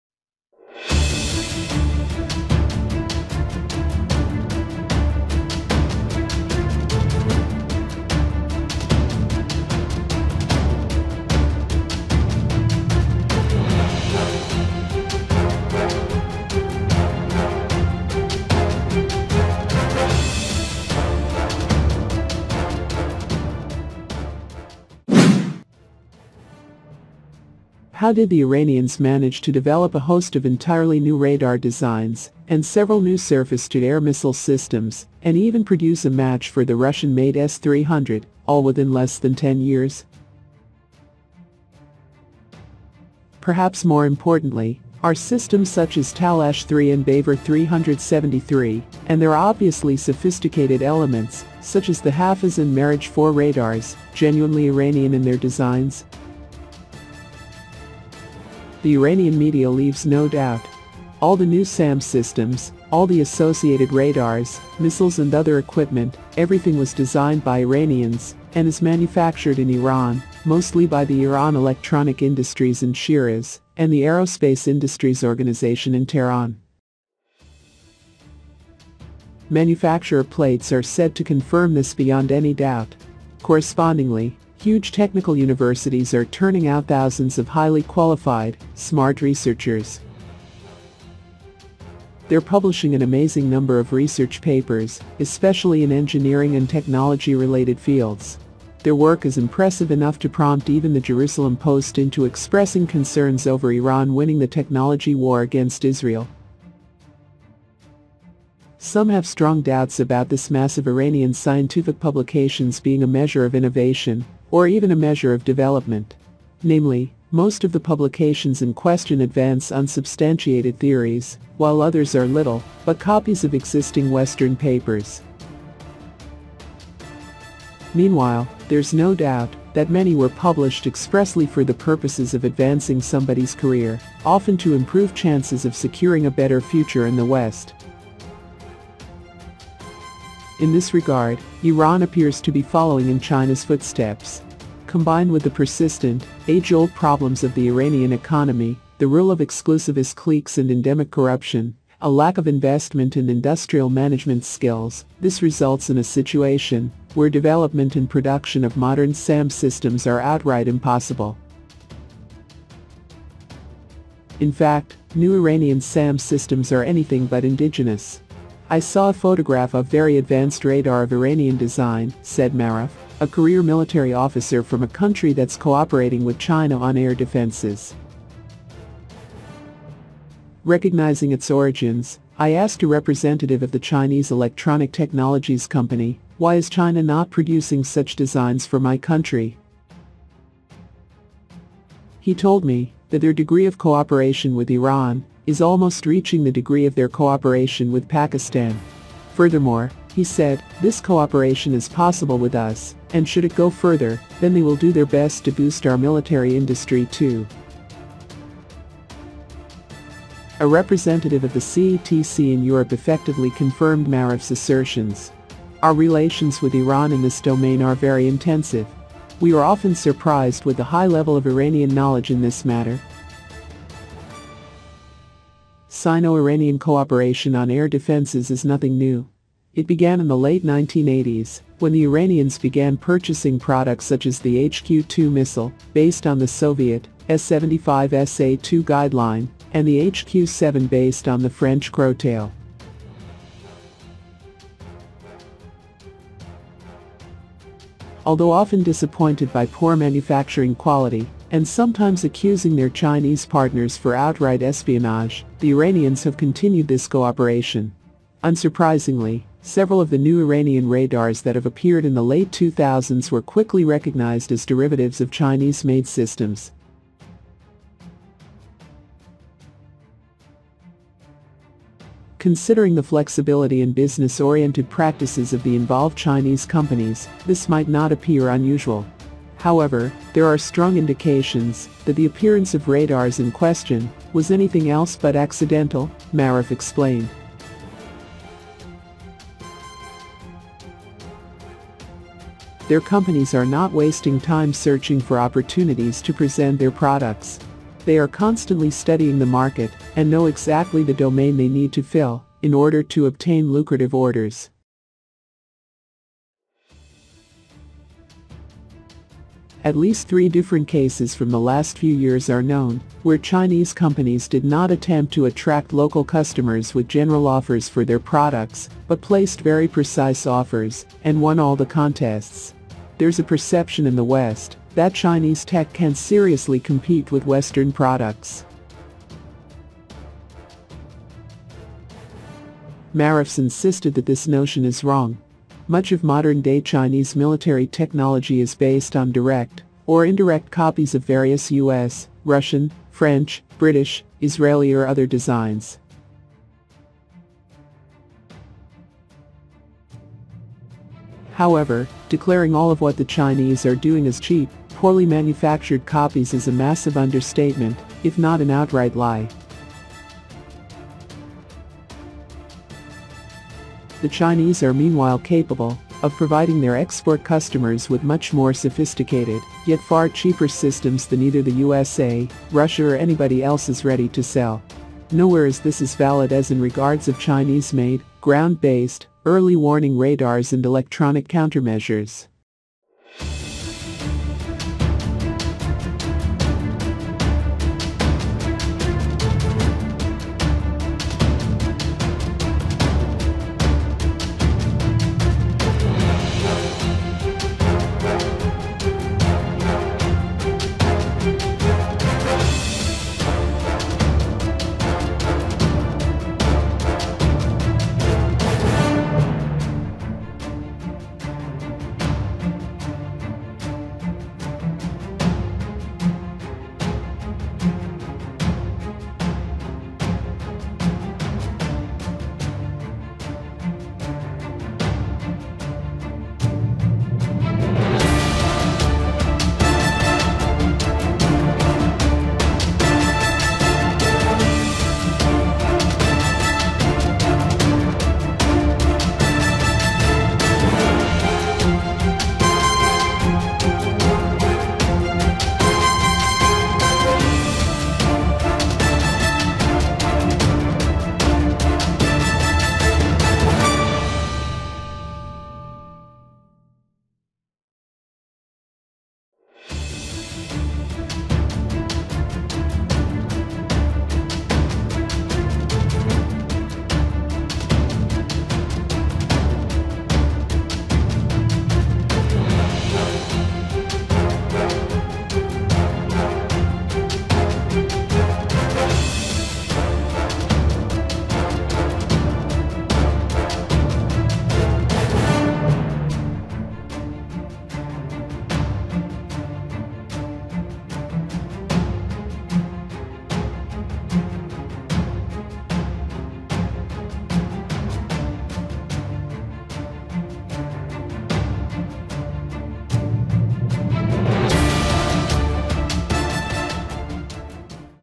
Iran Is Building Several New Deadly Surface-To-Air Missile Systems to Destroy Stealth Aircraft
How did the Iranians manage to develop a host of entirely new radar designs and several new surface-to-air missile systems and even produce a match for the Russian-made S-300 — all within less than 10 years?

The Iranian media leaves no doubt. All the new SAM systems, all the associated radars, missiles and other equipment — everything was designed by Iranians and is manufactured in Iran, mostly by the Iran Electronic Industries in Shiraz and the Aerospace Industries Organization in Tehran.